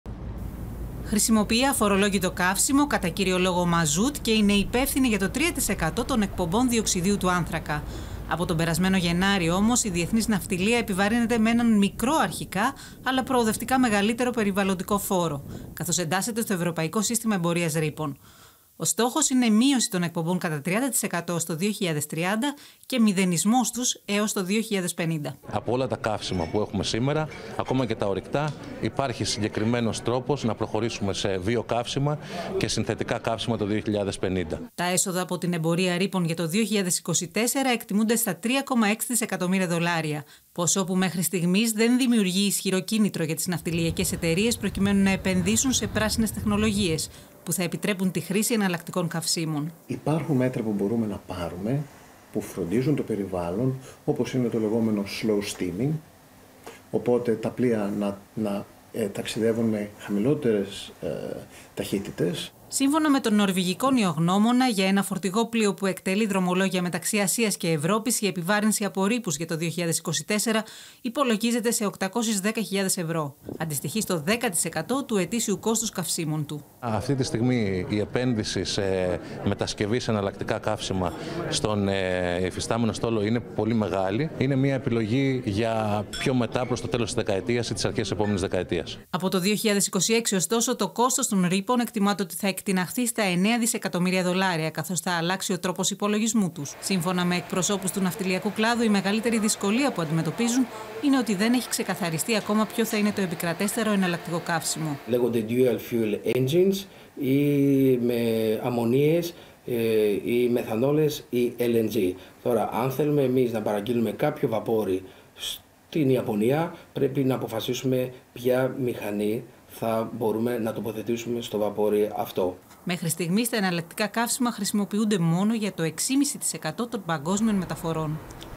Η πράσινη μετάβαση η σημαντικότερη οικονομική πρόκληση για τη διεθνή ναυτιλία
Η πράσινη μετάβαση η σημαντικότερη οικονομική πρόκληση για τη διεθνή ναυτιλία που εντάσσεται στο σύστημα εμπορίας ρύπων

ΕΙΔΗΣΕΙΣ ΟΙΚΟΝΟΜΙΑ
21/02/24 15:59
Ενημέρωση 13 ώρες πριν

Η πράσινη μετάβαση είναι η σημαντικότερη οικονομική και τεχνολογική πρόκληση που αντιμετωπίζει η διεθνής ναυτιλία. Ο Διεθνής Ναυτιλιακός Οργανισμός έχει θέσει στόχο την πλήρη απανθρακοποίηση των θαλάσσιων μεταφορών μέχρι το 2050, ενώ εδώ και 1,5 μήνα η ευρωπαϊκή ναυτιλία έχει ενταχθεί στο σύστημα εμπορίας ρύπων.

Θεόδωρος Κουρμπέλης, Strategic Business Development Partner, Lloyd's Register:
Από όλα τα καύσιμα που έχουμε σήμερα, ακόμα και τα ορυκτά, υπάρχει συγκεκριμένος τρόπος να προχωρήσουμε σε βιοκαύσιμα και συνθετικά καύσιμα έως το 2050.
Αυτή τη στιγμή η επένδυση σε μετασκευή σε εναλλακτικά καύσιμα στον υφιστάμενο στόλο είναι πολύ μεγάλη. Είναι μια επιλογή για πιο μετά, προς το τέλος της δεκαετίας ή τις αρχές της επόμενης δεκαετίας.